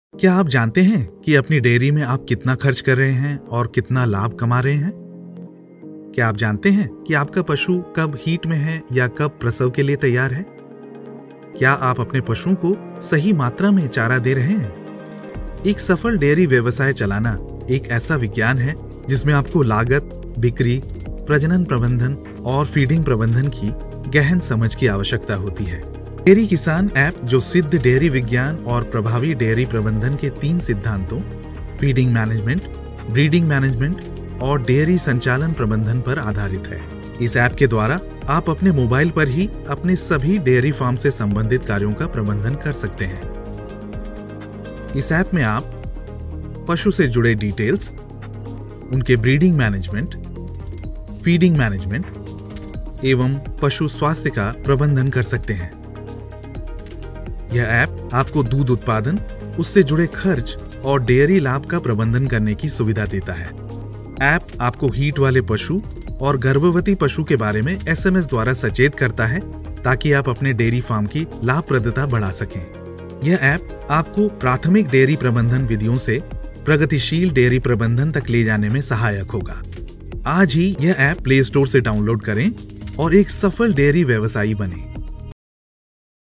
Dairy Kisan - A next gen Dairy Management App
Dairy Kisan is a revolutionary next gen dairy management app where you can manage all your dairy farm related functions on your mobile app. This app allows you to manage animal profiles, breeding, feeding, milk production, animal health history, and your dairy profit and loss. The app also alerts you about the animals in heat, animals that are pregnant and animals that are repeating so that you can manage the profitability of your farm.